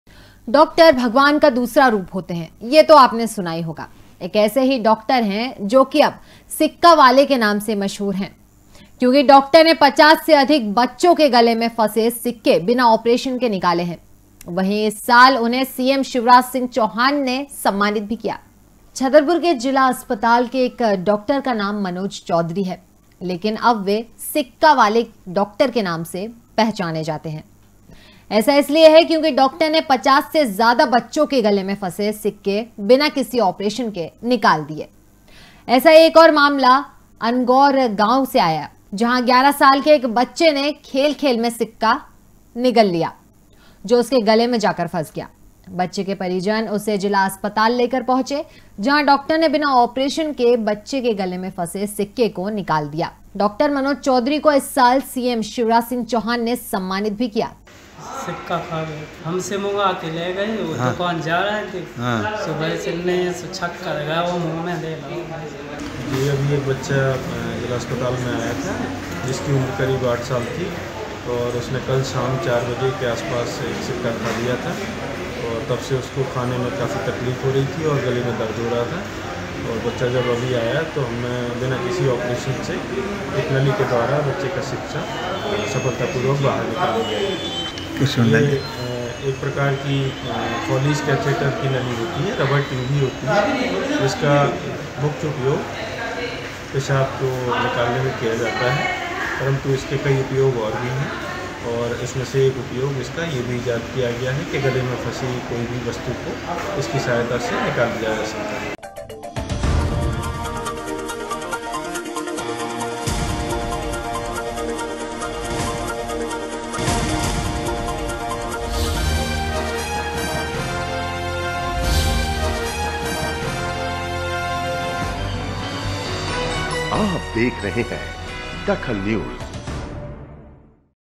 DAKHAL NEWS बिना ऑपरेशन निकाले गले में फसे सिक्के
बिना ऑपरेशन निकाले गले में फसे सिक्के
अब सिक्के वाले डॉक्टर के नाम से मशहुर